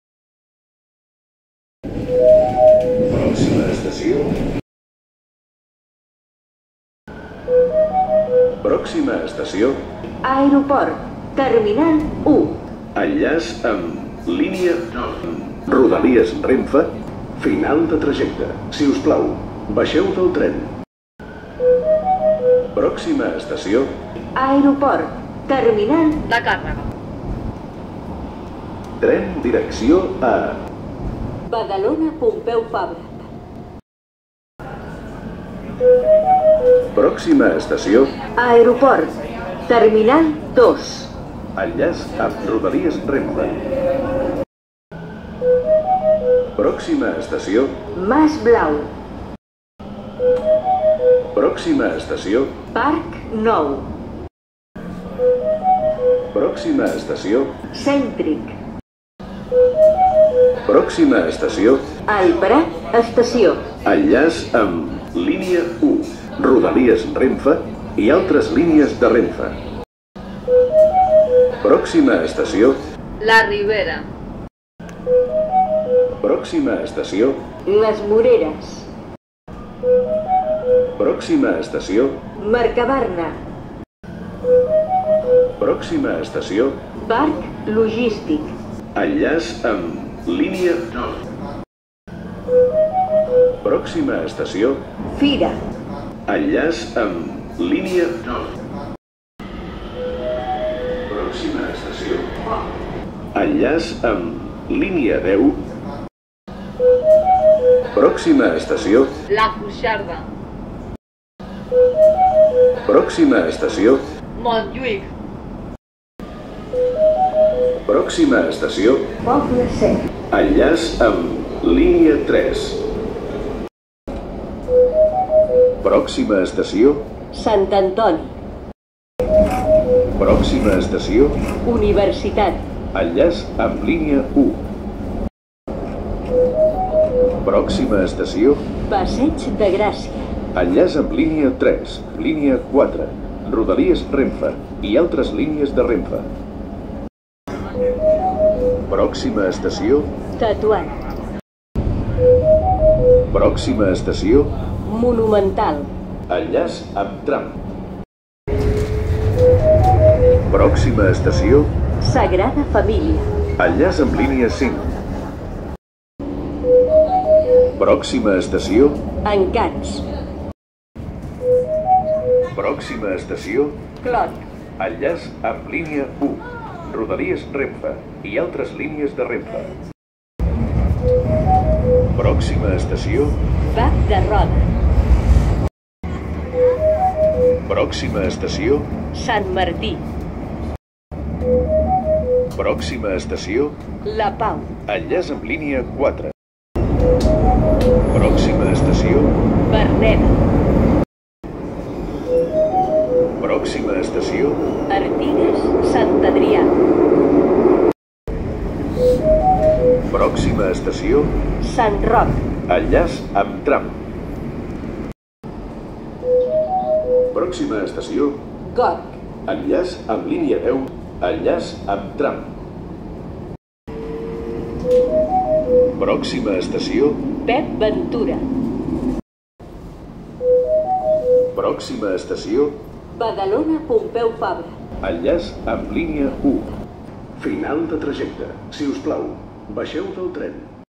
Megafonia L2 Metro de Barcelona Aeroport T1 - Badalona Pompeu Fabra FUTUR | Sèrie 9000
Simulació de com serà la megafonia (versió en català) de la línia 2 del metro d'aquí uns anys, amb l'ampliació fins l'aeroport passant per Montjuïc i fent servir part del tram de la L9 pel Prat de Llobregat.